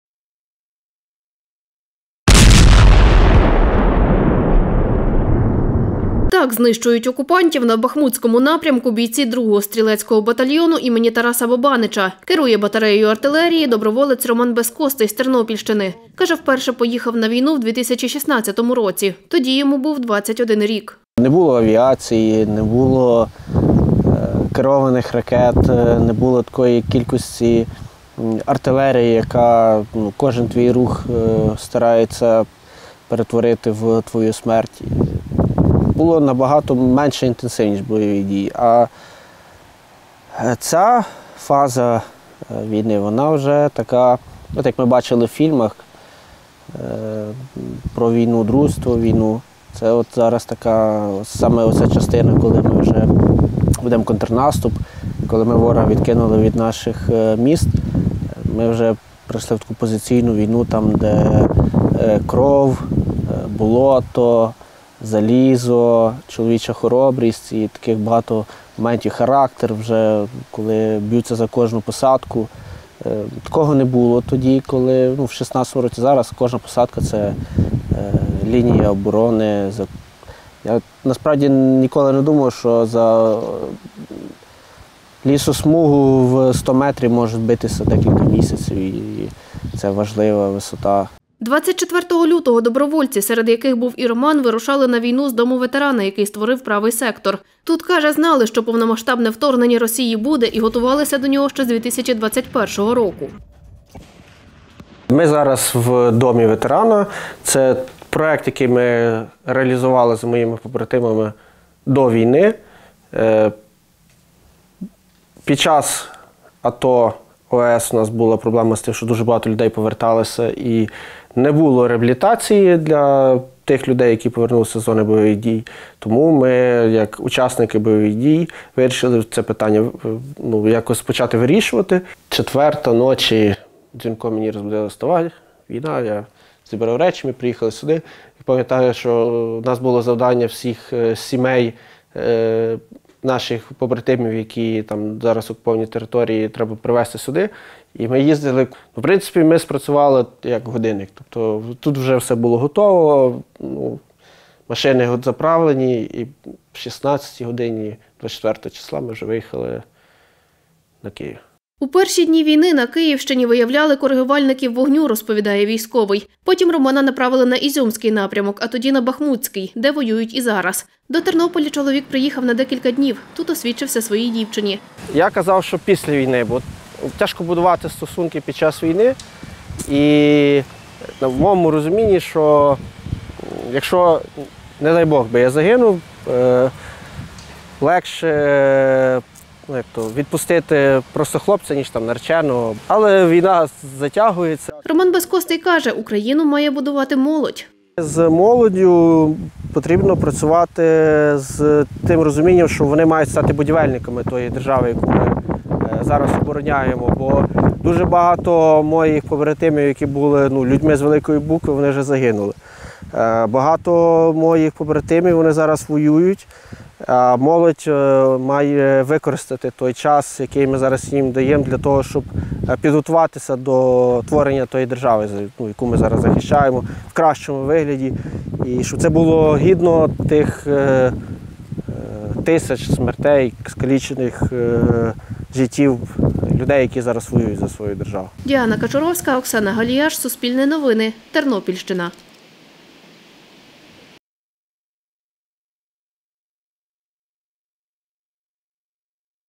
Командир батареї 2 стрілецького батальйону ім. Тараса Бобанича Роман Безкостий розповів свою історію
«Ми даємо час молоді підготуватися до будівництва держави» – так про російсько-українську війну каже доброволець, командир батареї 2 стрілецького батальйону імені Тараса Бобанича Роман Безкостий. Чоловік розповідає, боротьбі за Україну присвятив усе своє свідоме життя, як і його предки-упівці. Про сучасну війну та що робитиме після перемоги – розповів Суспільному.